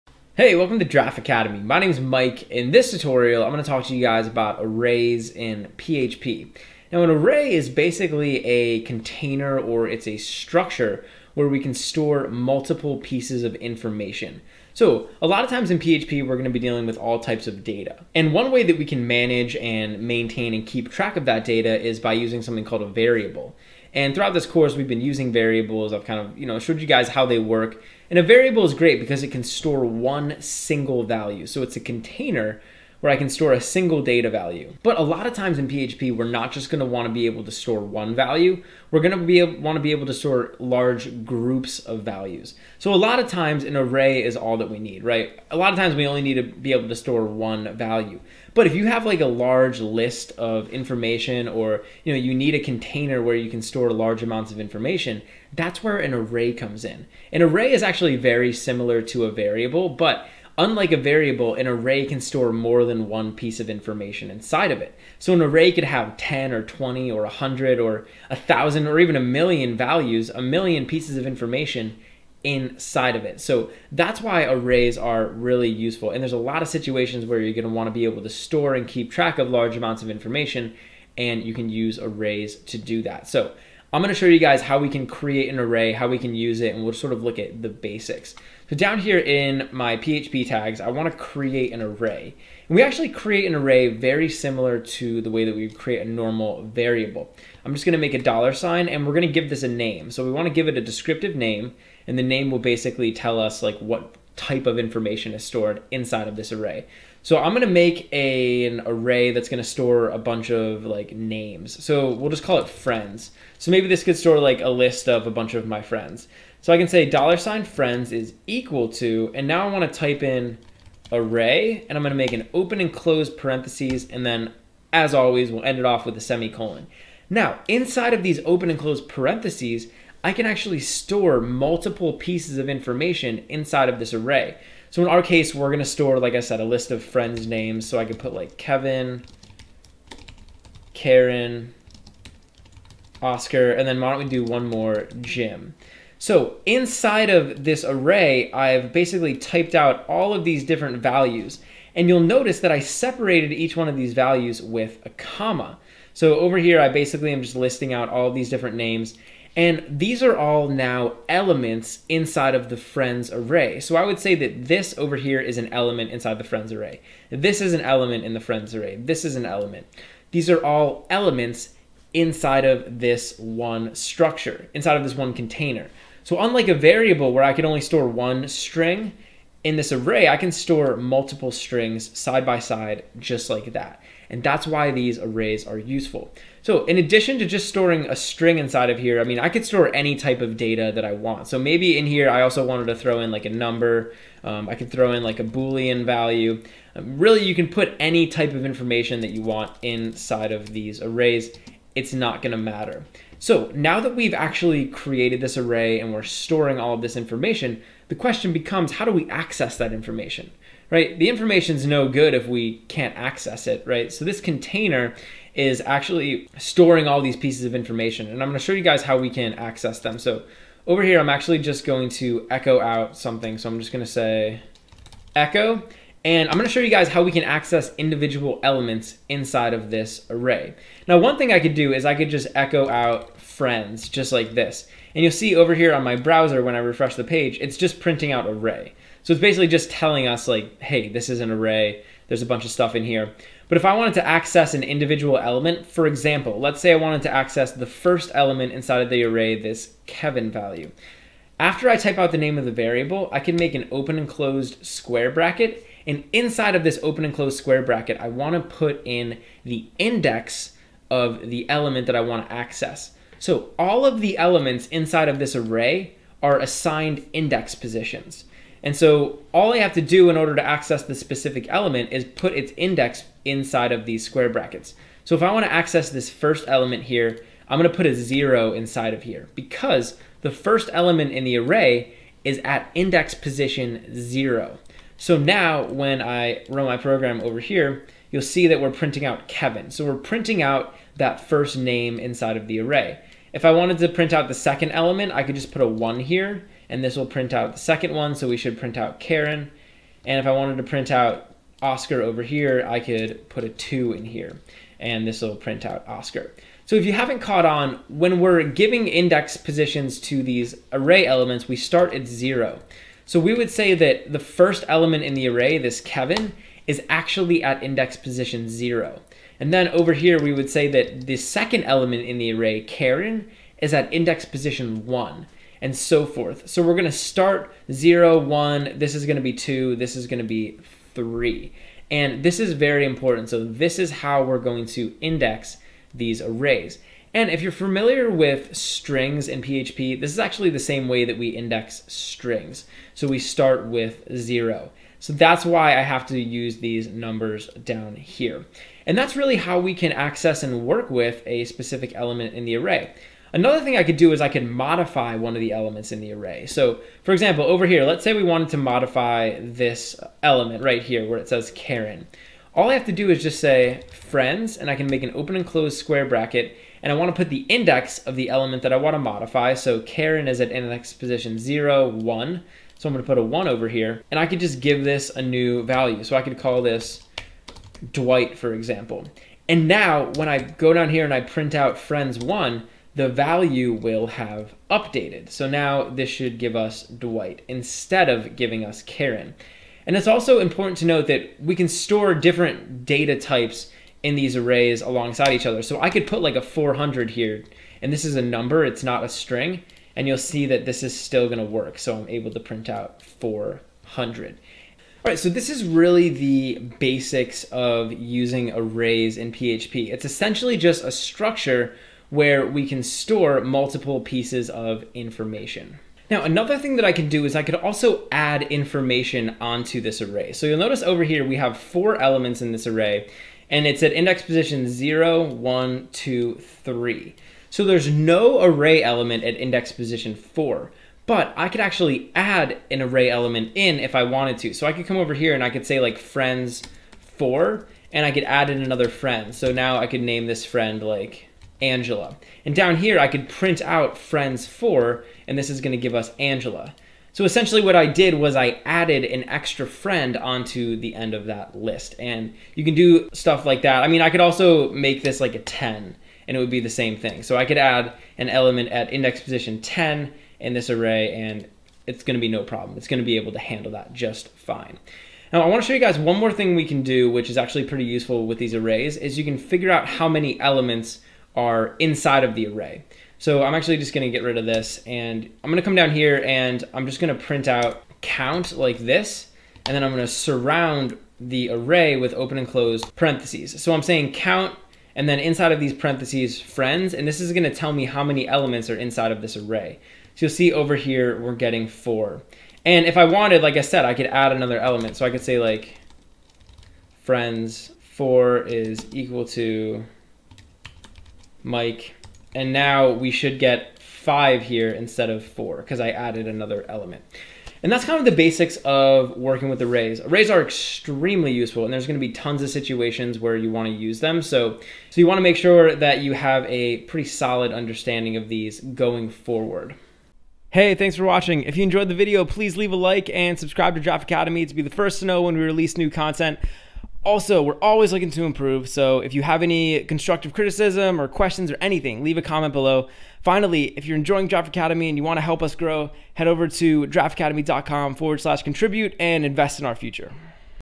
Arrays | PHP | Tutorial 15
This video is one in a series of videos where we'll be looking at programming in PHP. The course is designed for new programmers, and will introduce common programming topics using the PHP language.

If you’re enjoying Giraffe Academy and want to invest in our future, consider leaving a contribution